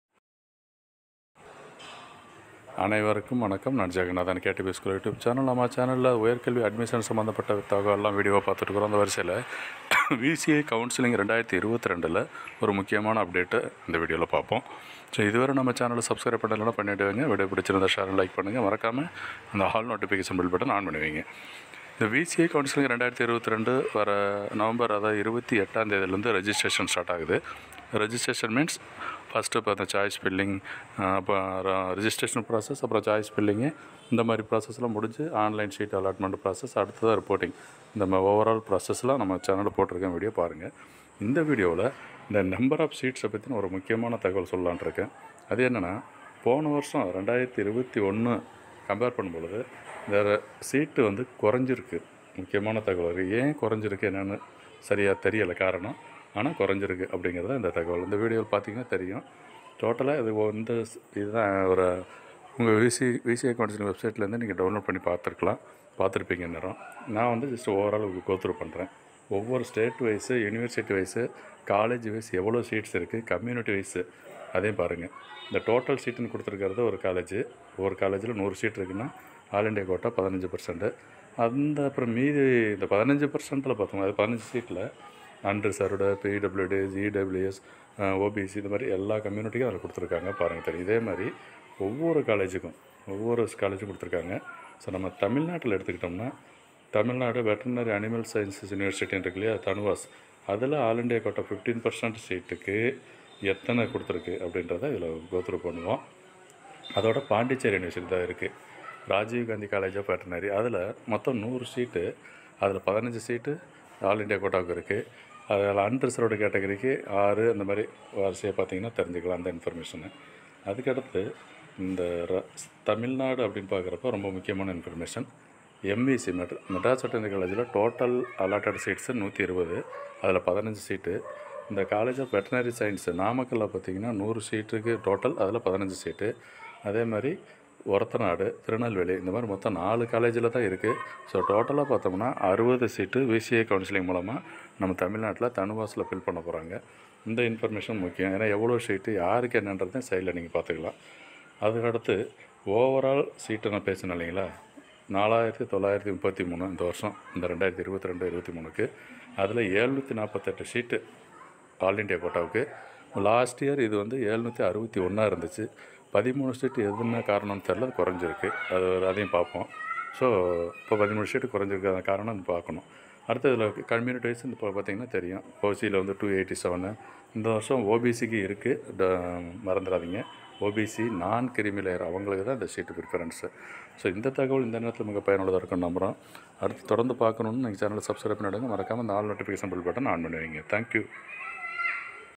VCI COUNSELLING 2022 | Seat Matrix | Seats Reduced, why? | Nov 28-Dec 9 | BVSc & AH (English/தமிழ்)
KTVSchool VCI tanuvas VCI COUNSELLING 2022 | Seat Matrix | Seats Reduced, why? | Nov 28-Dec 9 | BVSc & AH (English/தமிழ்)

You can find all Higher Education Admission-related latest information in video format.